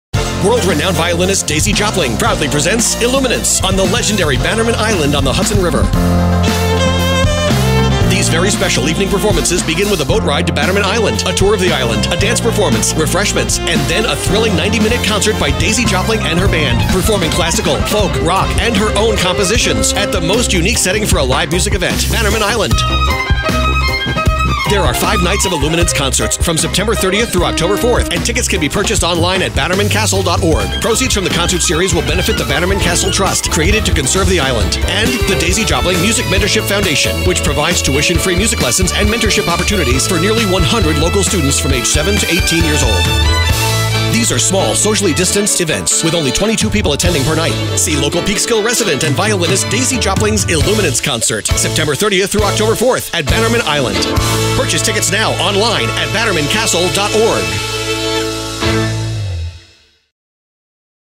Daisy Jopling Foundation & Bannerman Island Trust Present: "Illuminance" (Radio Commercial)
"Illuminance" is now on the airwaves! Check out our radio commercial for the series of shows taking place on Bannerman Island from September 30th-October 4th

to reserve you spot today!